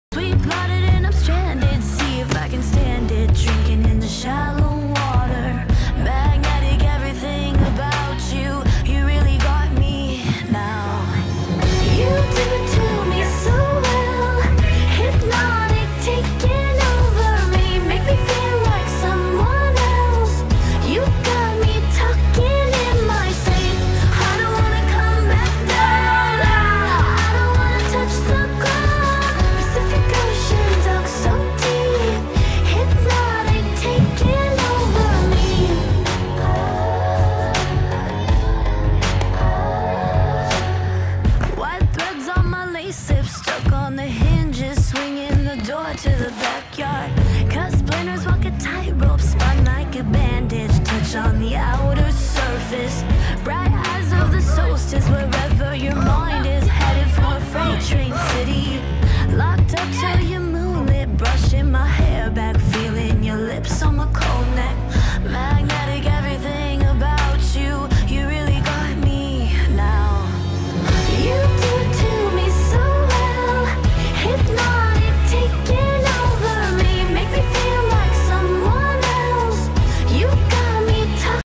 Tumbling ep.1
Im doing a series of tumbling episodes . Each episode will come of once i get new or improved skills! This included specailty passes to full, standing fulls, doubles, arabian and kickfulls, and more!